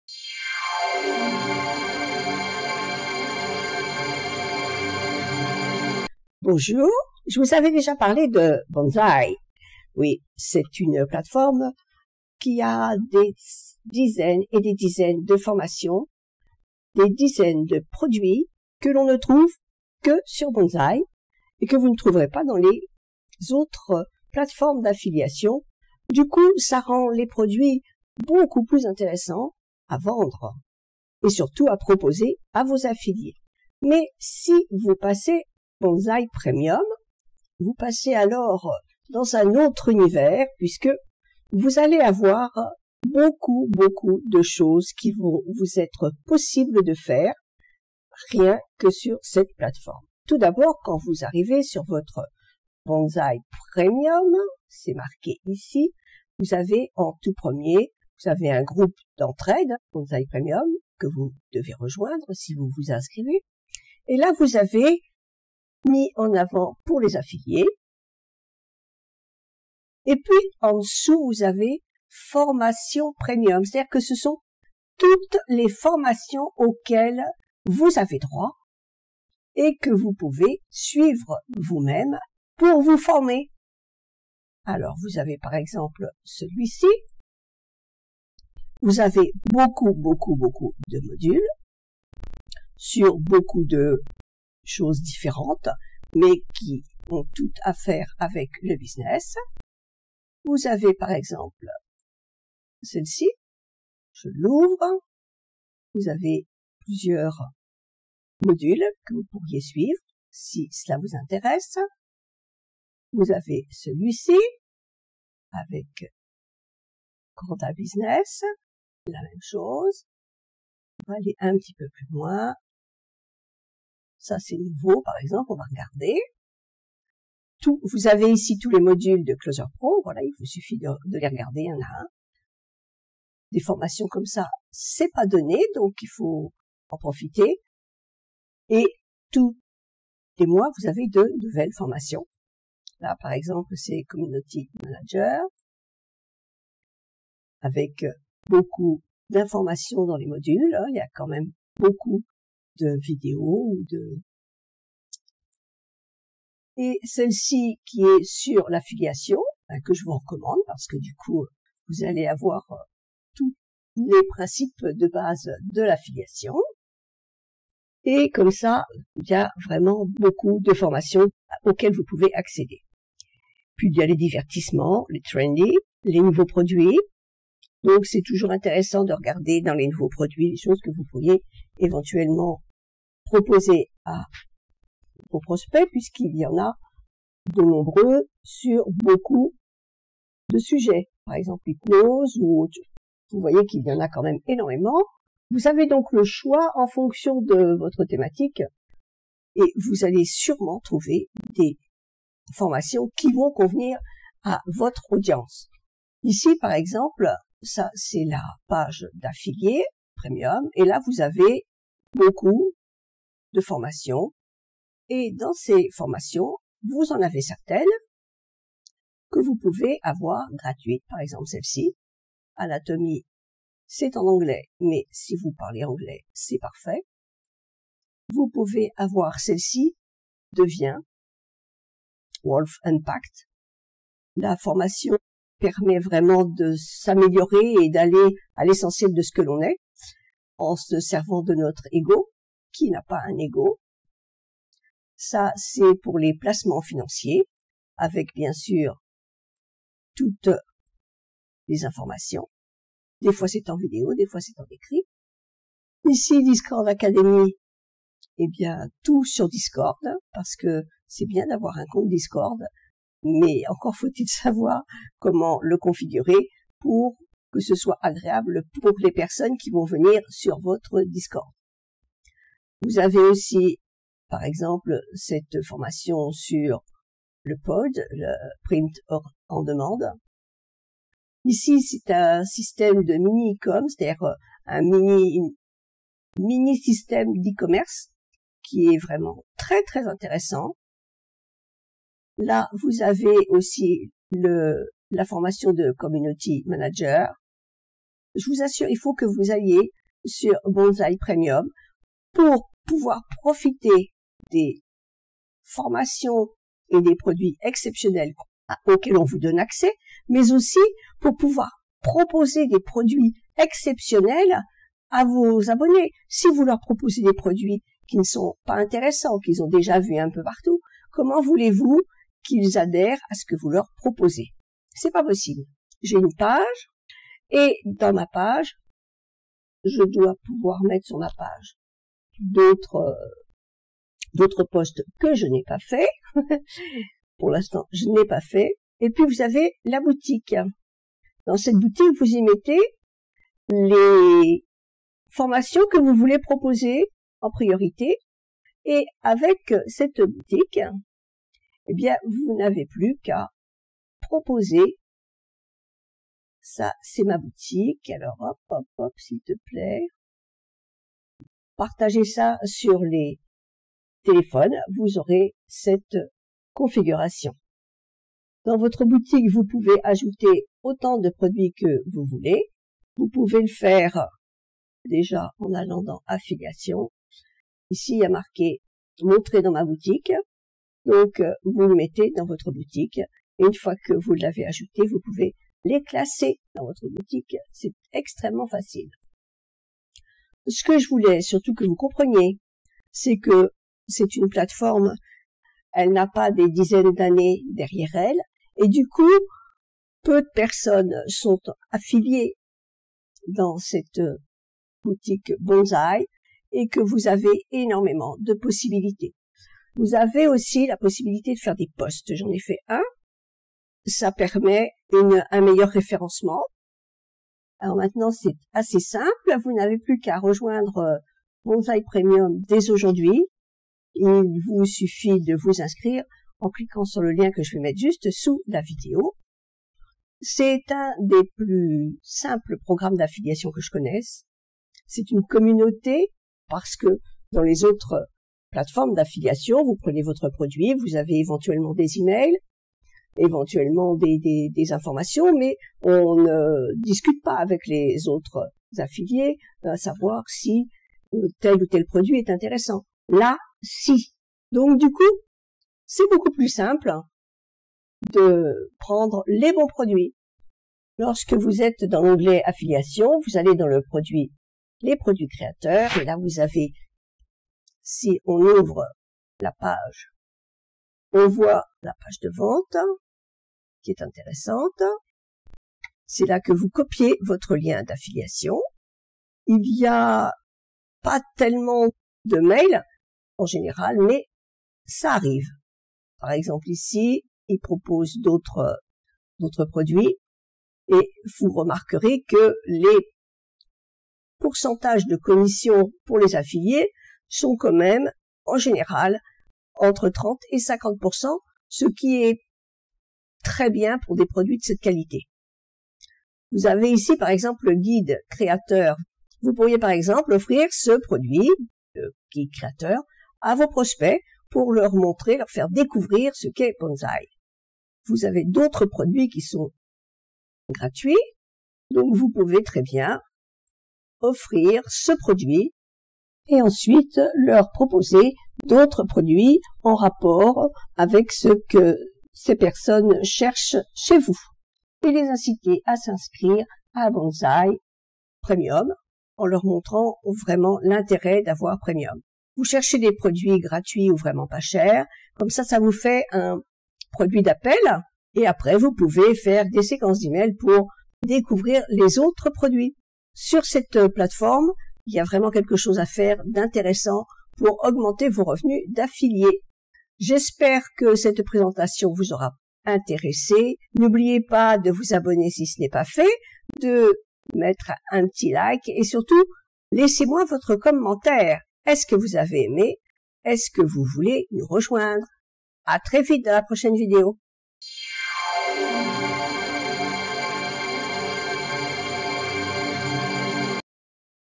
Qu'est-ce qui fait de Bonzai la MEILLEURE plateforme d'affiliation pour 2024
Dans cette vidéo, Pourquoi Bonzai écrase les autres plateformes d'affiliation en 2024, découvrez tout ce que vous devez savoir sur les avantages de Bonzai Premium en 2024. Vous accédez à du contenu que vous ne trouverez nulle part ailleurs, idéal pour les affiliés cherchant à se démarquer.
🔑 Les points forts de Bonzai Premium :
Formations renouvelées : Accédez à des modules mis à jour régulièrement, garantissant des compétences toujours à jour.
Produits exclusifs : Bénéficiez d’offres uniques, introuvables sur d’autres plateformes.
🎯 Pourquoi Bonzai Premium est un choix incontournable ?
Bonzai Premium permet aux professionnels du marketing d’affiliation d’avoir un accès continu à des formations actualisées et à des produits exceptionnels, augmentant ainsi leur potentiel de revenus. En 2024, il est l'outil indispensable pour booster vos performances et élargir vos opportunités.
Ne manquez pas cette occasion de découvrir comment Bonzai Premium peut transformer votre approche de l’affiliation. Cliquez